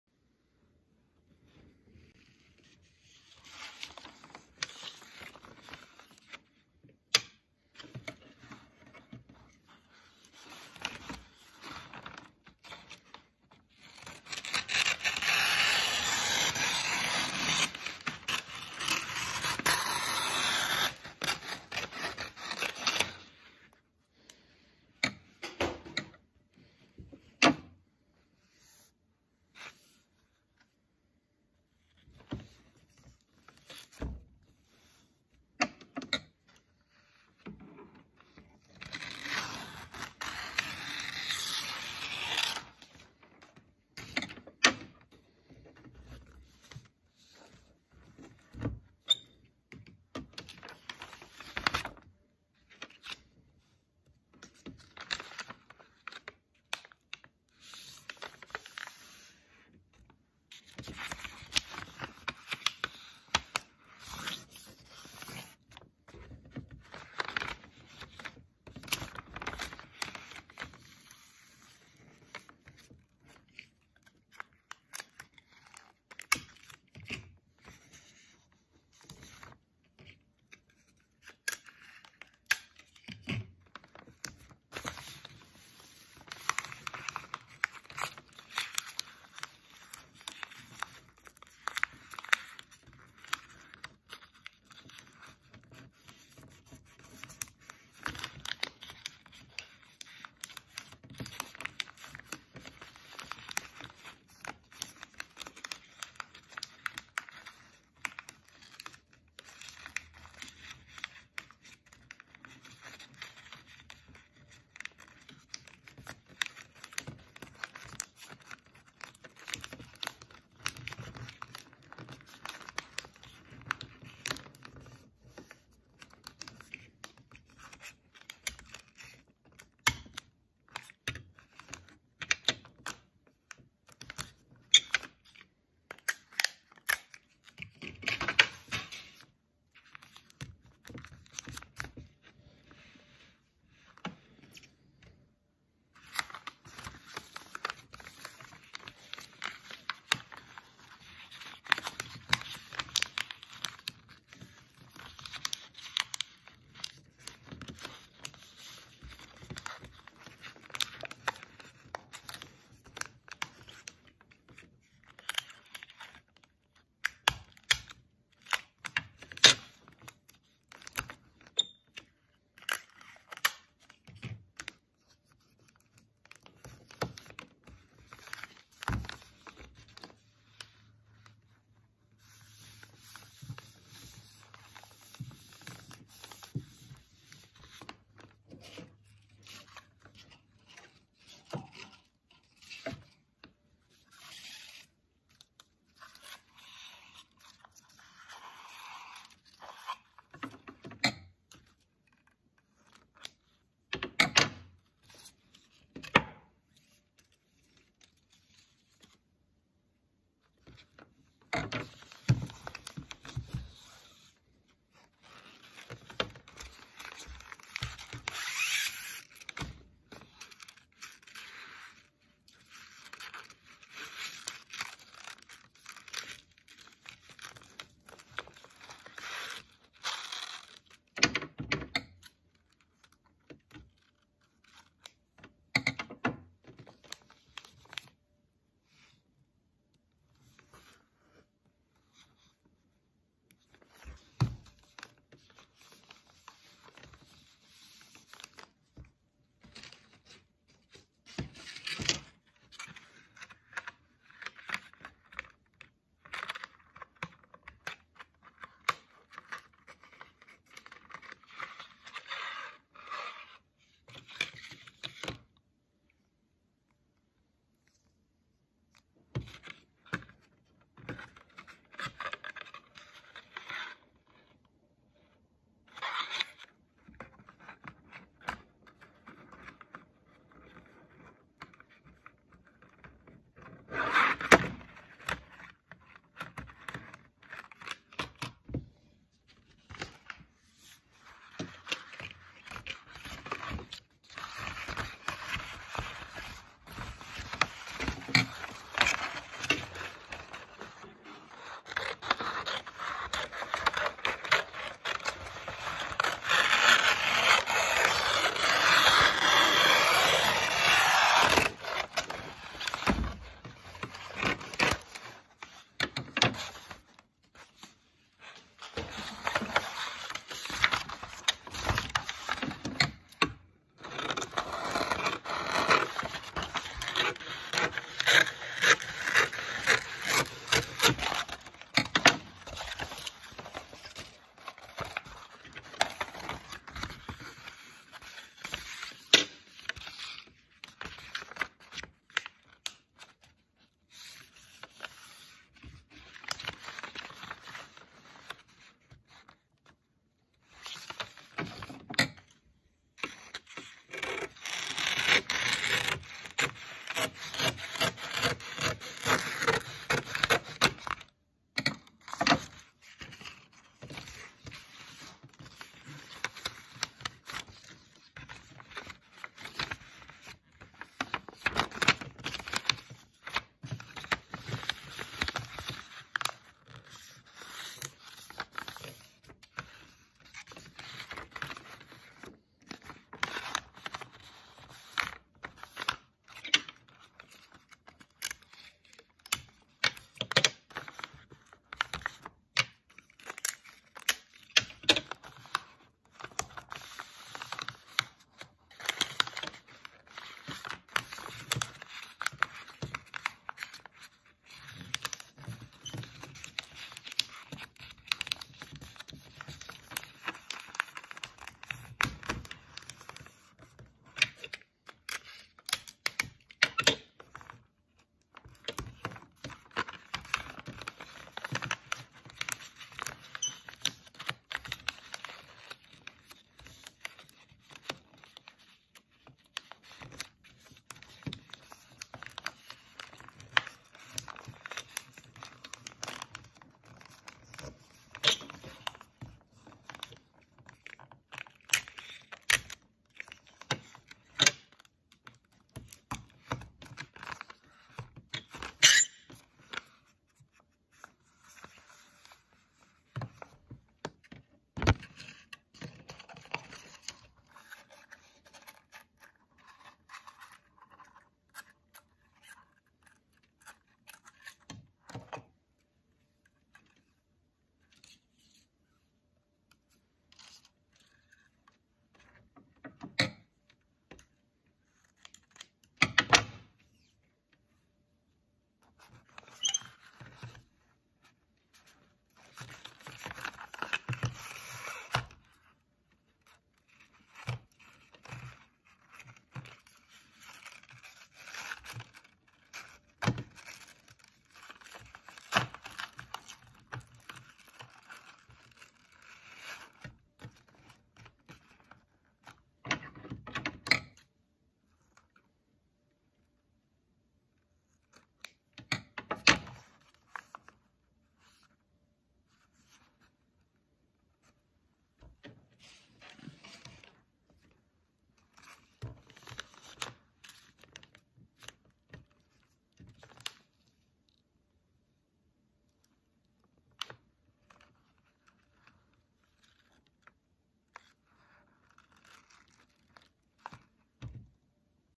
Claire’s ASMR Gift wrapping 🎁 Showing how to wrap a book 📖 markers 🖍 as gifts 💝ribbon
Hi everyone 🎀 using a red ribbon to wrap a book and a box of whiteboard markers in different colors 🎁💝🎀 2 gifts 💛